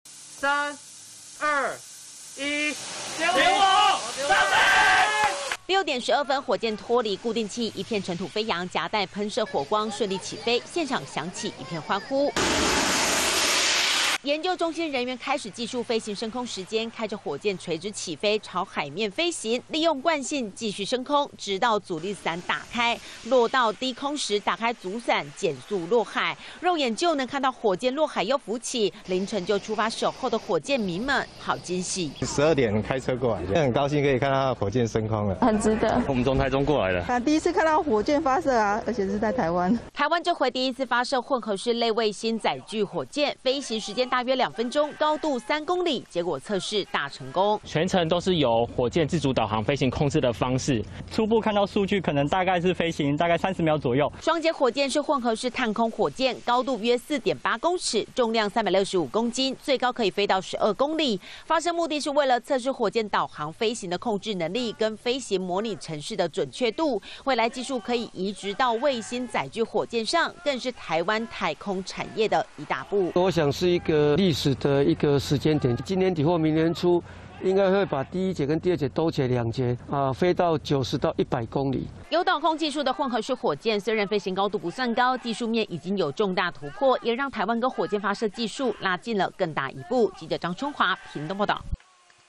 大成功 ! 全台首枚"科研火箭" 旭海升空"歡聲雷動"│中視新聞 20220710
屏東旭海在今天早上六點12分成功發射台灣第一支混合式類衛星載具火箭原本預計是5天前因為遇到風雨取消又從昨天延到今天終於順利發射升空也讓現場圍觀群眾連工作人員都歡聲雷動。